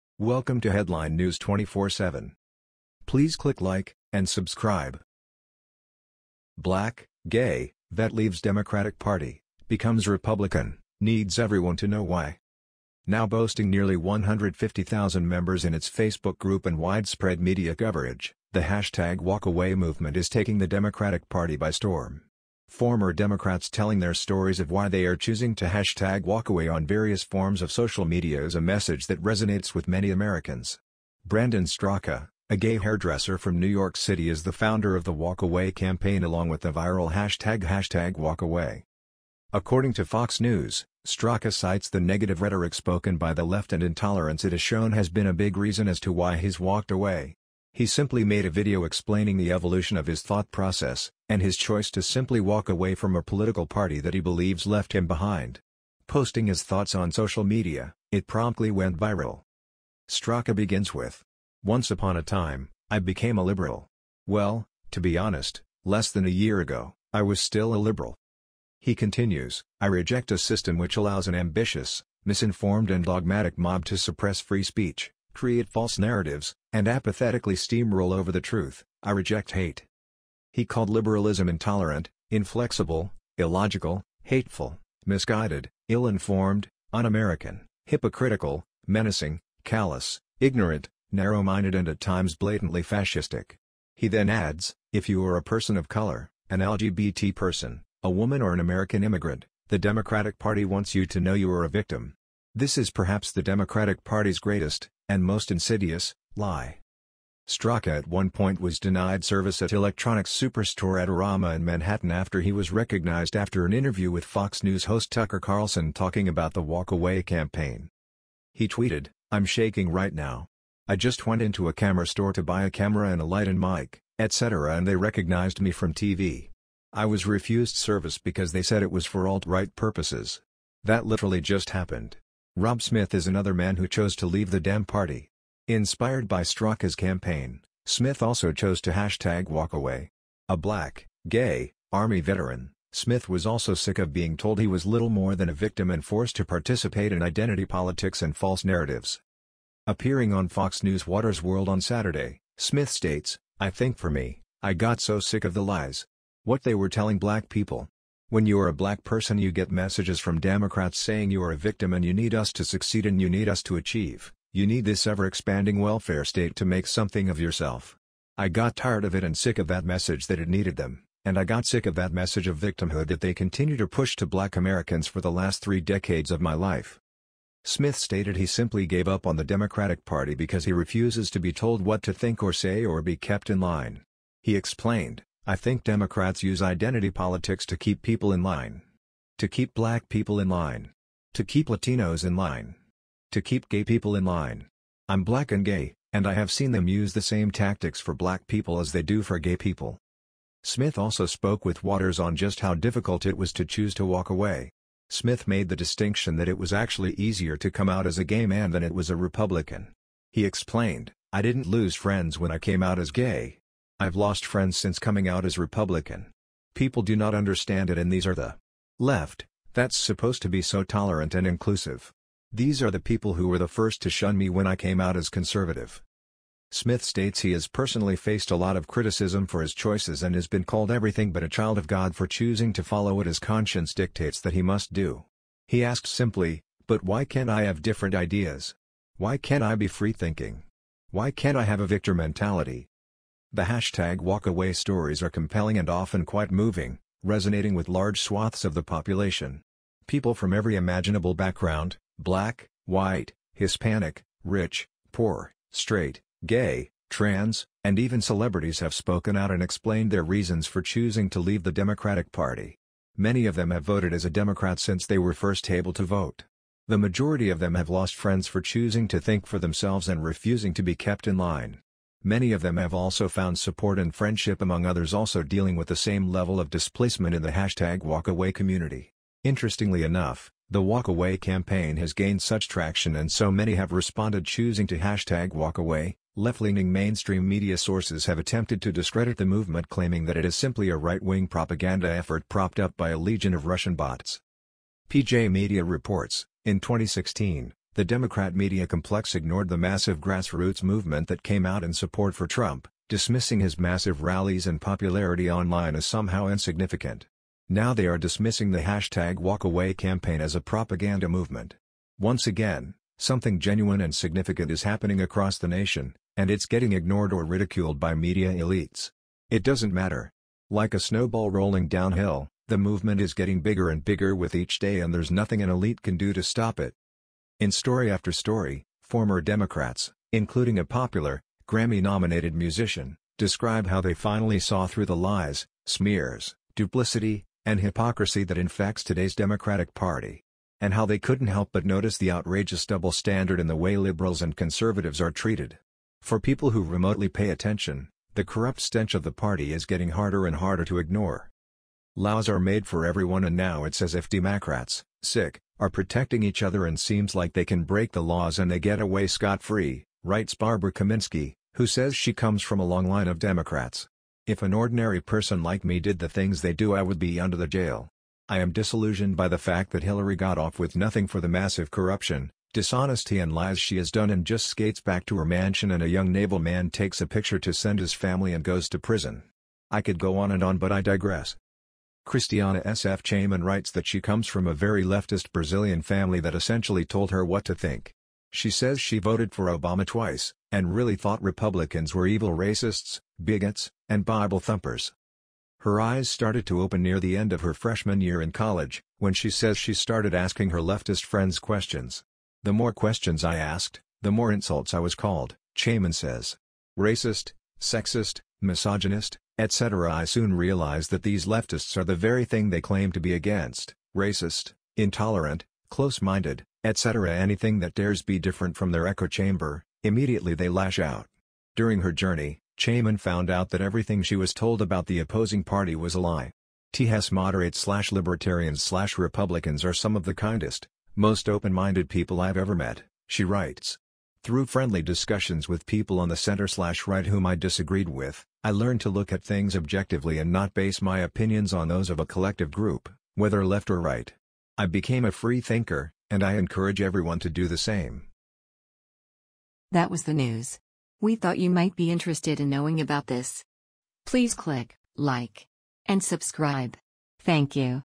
Black, Gay, Vet Leaves Democratic Party, Becomes Republican – Needs Everyone To Know Why
Now boasting nearly 150,000 members in its Facebook group and widespread media coverage, the WalkAway movement is taking the Democratic Party by storm. Former Democrats telling their stories of why they are choosing to walkaway on various forms of social media is a message that resonates with many Americans.